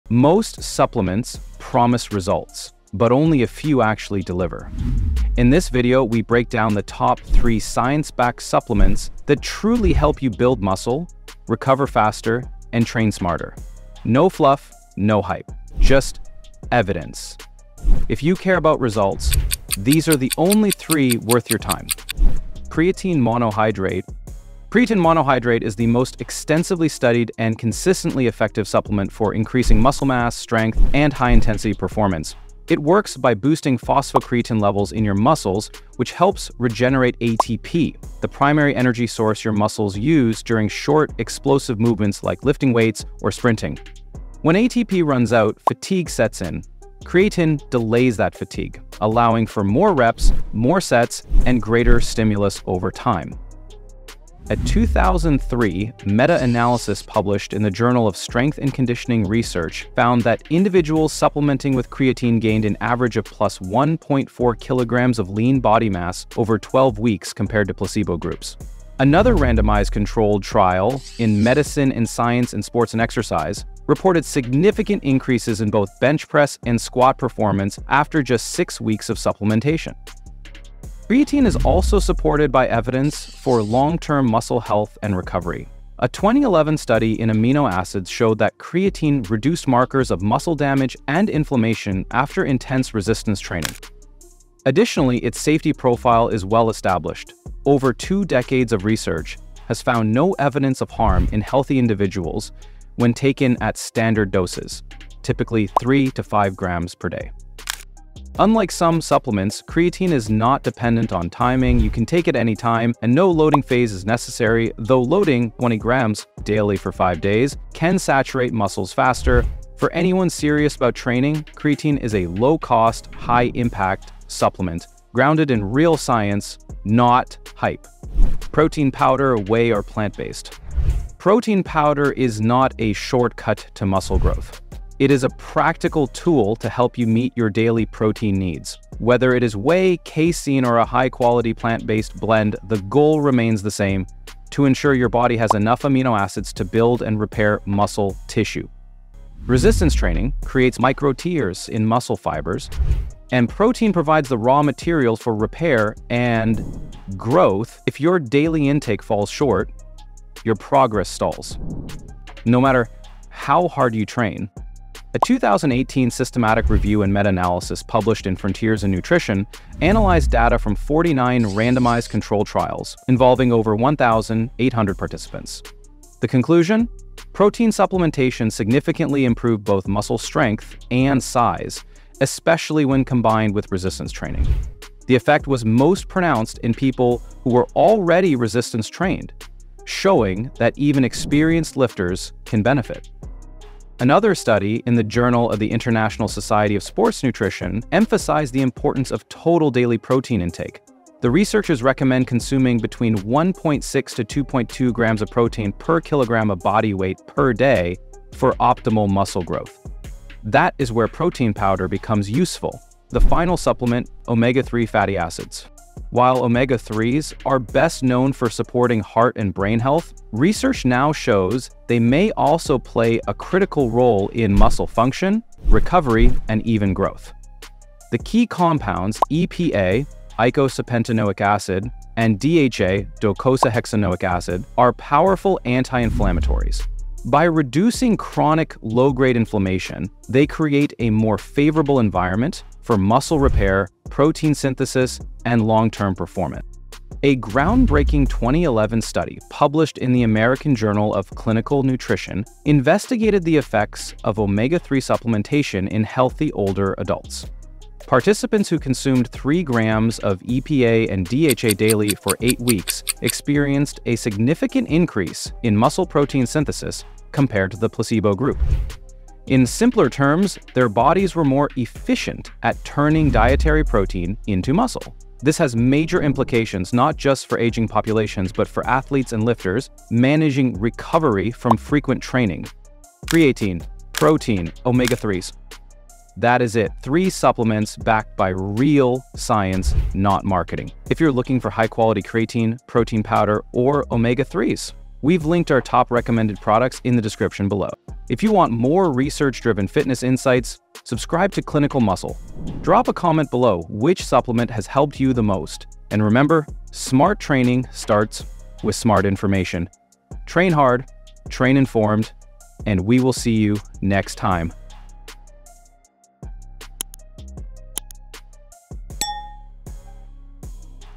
Top 3 Supplements for Muscle Growth
Looking for the best supplements to build muscle faster?
Most supplement advice is full of hype — but in this video, we break down the top 3 supplements for muscle growth backed by real science.

Whether you're an experienced lifter or just starting out, these supplements can help you:
✅ Build lean muscle mass
✅ Recover faster after workouts
✅ Train harder with better output

🔗 Our Recommended Supplements
These are clean, science-backed options that reflect the research covered in this video:
✅ Creatine Monohydrate – Backed by 20+ Years of Data
✅ Whey Protein Powder – High-Quality, No Fillers
✅ Omega-3 Krill Oil – For Recovery & Inflammation

🎓 What You’ll Learn in This Video:
✅ Why creatine monohydrate is the most effective supplement for increasing strength, lean mass, and high-intensity performance
✅ How protein powder (whey or plant-based) helps fill nutritional gaps and supports muscle repair after training
✅ The overlooked power of omega-3s (EPA & DHA) to reduce inflammation, support muscle protein synthesis, and improve recovery
✅ Research-backed dosing, timing tips, and what actually matters

🧠 Backed by Real Studies:
This video references peer-reviewed studies and meta-analyses — including research from the Journal of Strength and Conditioning Research, Frontiers in Nutrition, and the American Journal of Clinical Nutrition.
We cut through the noise and give you what actually works.

💬 Drop a comment: Which of these 3 supplements has helped you the most?